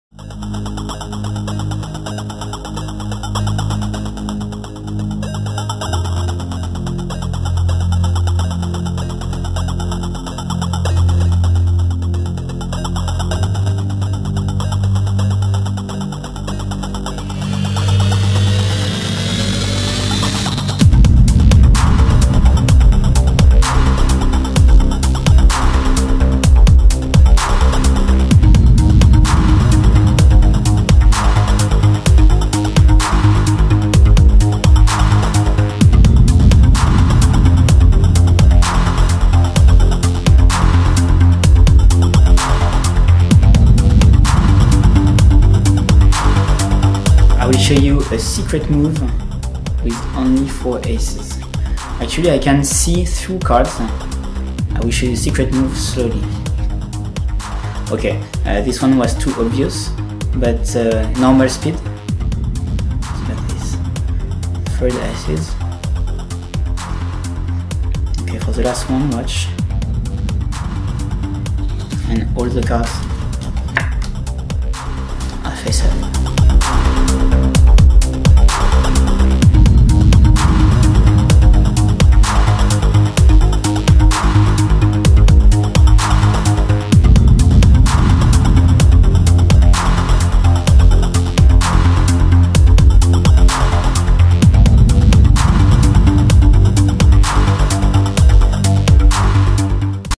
Wavelength by Manu Sura
'Waving the Aces' by Guy Hollingworth is one of the most famous and deceptive pieces of card magic ever created. The effect is simple. Four aces are shown face down, one by one they visually turn face up.

For many years Manu Jo has studied 'Waving the Aces', along the way discovering new sleights which allow for a more visual and deceptive handling that is truly baffling to both spectators and magicians alike.

Manu Jo's handling of this classic of card magic is guaranteed to leave a lasting impression in the mind of your spectators. We are delighted to share his work with you. 'Wavelength' features five impromptu routines, with skill levels ranging from beginner to expert, all done with a borrowed deck and with no angle restrictions.

Tricks :

Red : This is the 'basic' handling that started it all

Yellow : 2 aces turn face down, one at a time in a spread of four cards, you turn this packet over and the two other aces turn face  ...